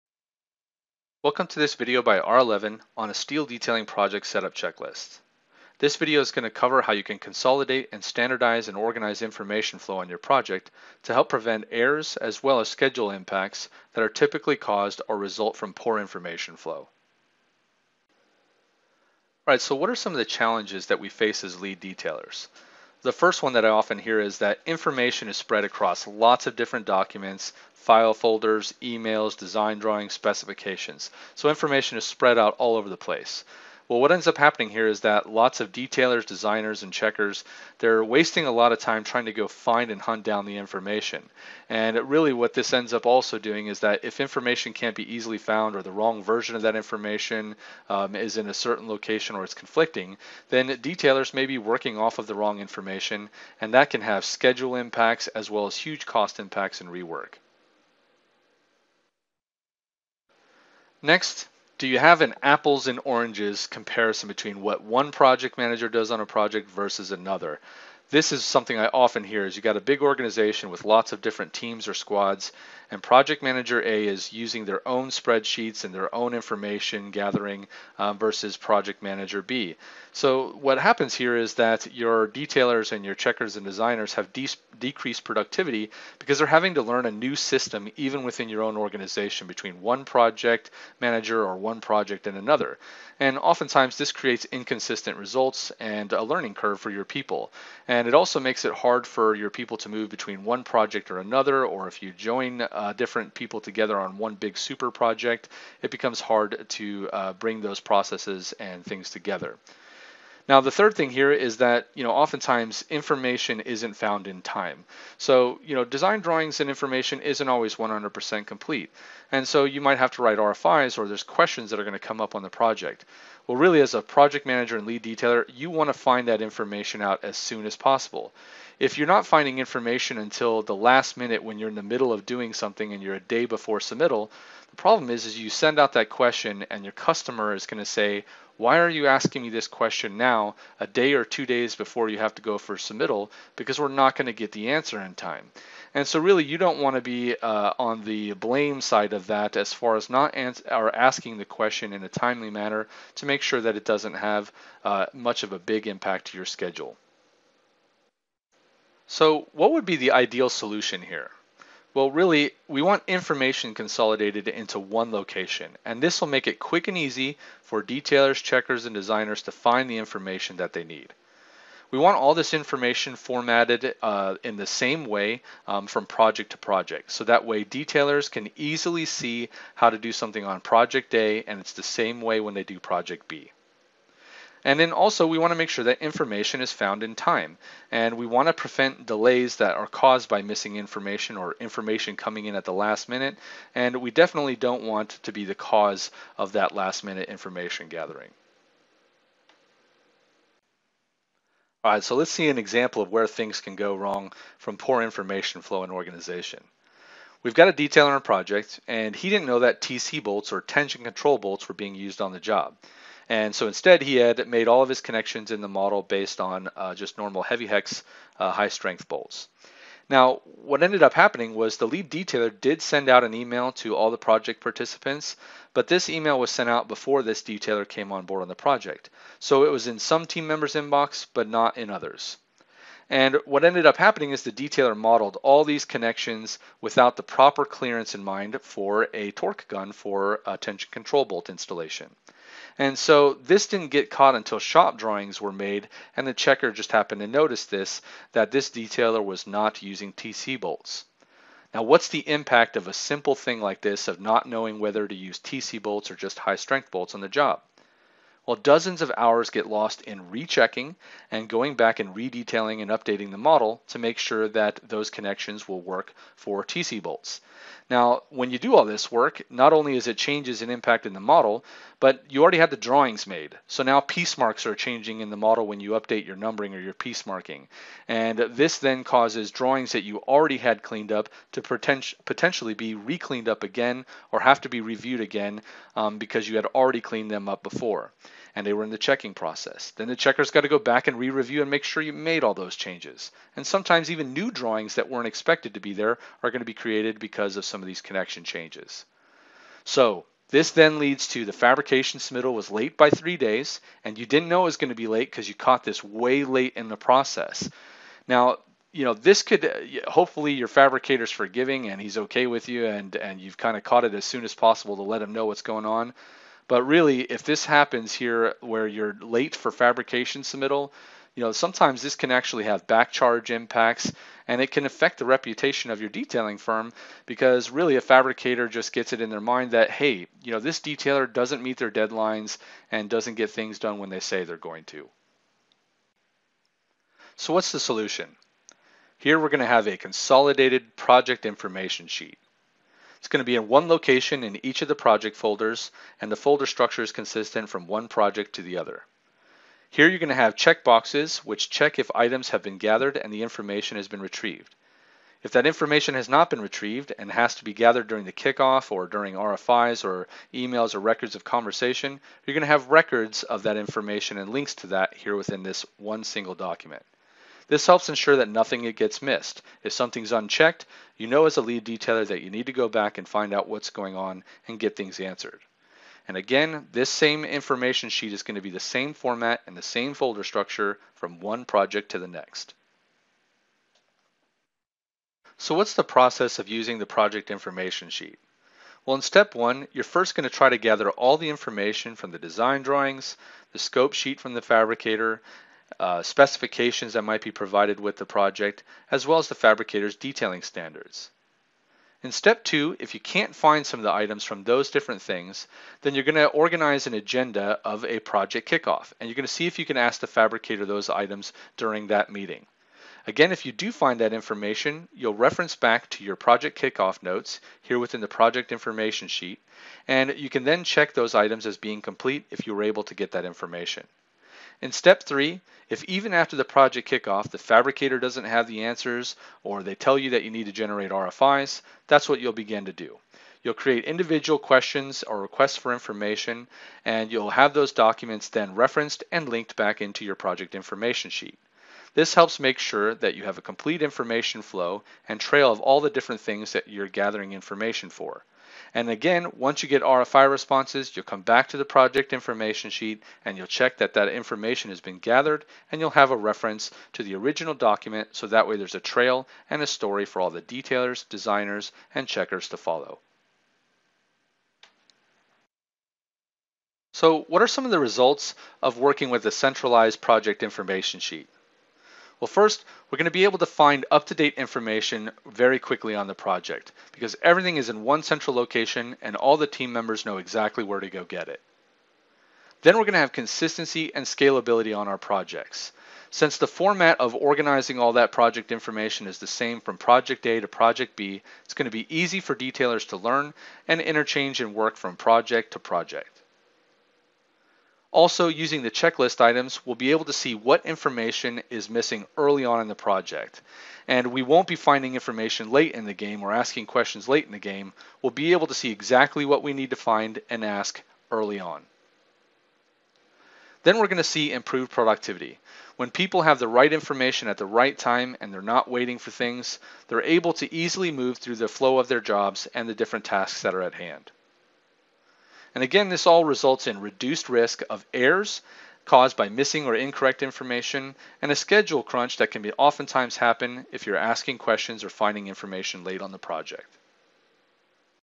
ARE11: Why Steel Detailers Need a Project Checklist
Keeping organized and informed early on an in a project is critical to staying on schedule and preventing errors as a steel detailer. You don't want to be held up in production because you asked a last minute RFI, or redetailing work because a detailer didn't know you were using TC bolts on the job. Check out this video to see why Detailing kick off checklists and a centralized project information sheet are key to a successful project.